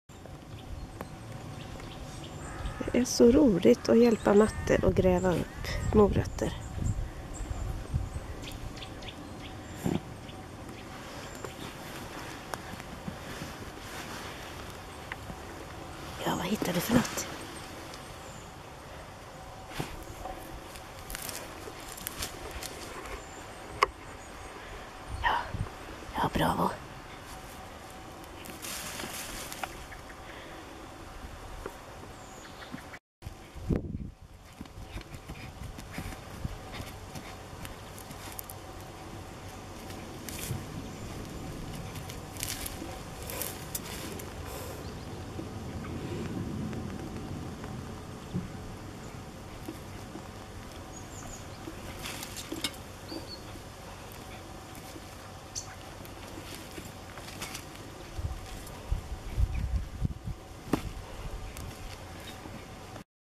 Sigge gräver upp morötter
Det är bra med en hjälpreda i trädgården. Dessutom tyckte han det var så roligt.